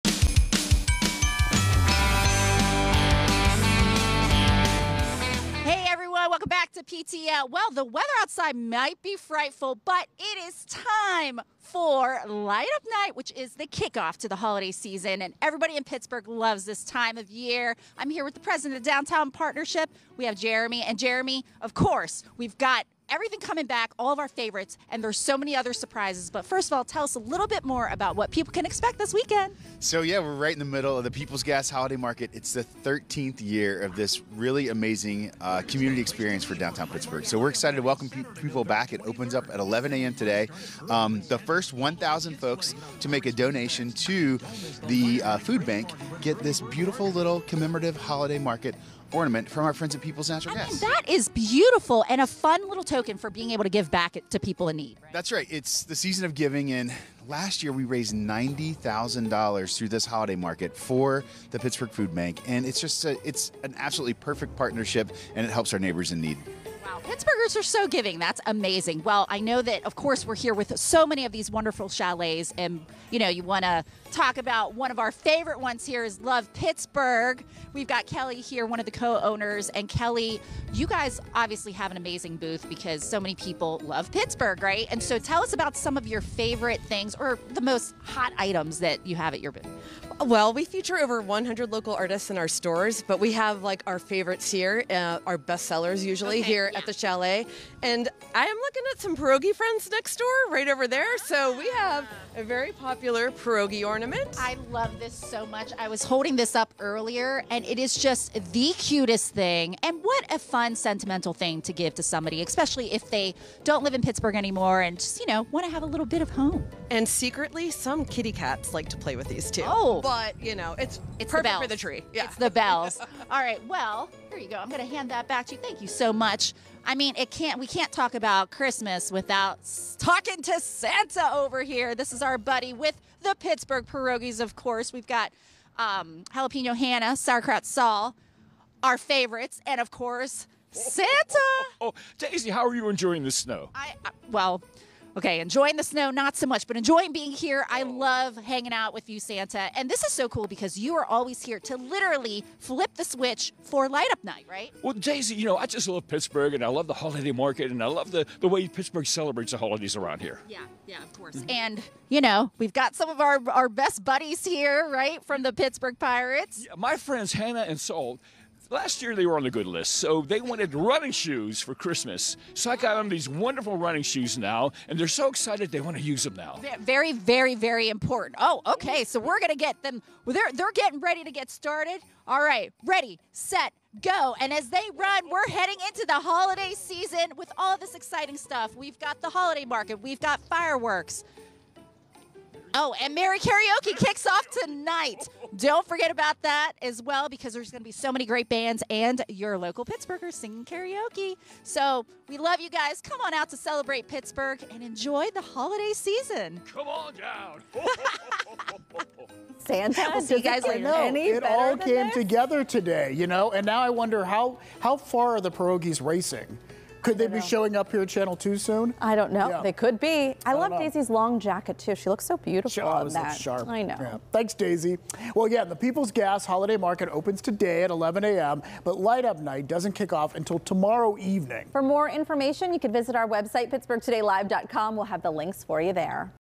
PTL gets a preview of Light Up Night weekend
Thousands of yinzers are getting ready to descend on downtown Pittsburgh to kick off the holidays. Daisy Jade joined us from Market Square with a preview of the star-studded lineup.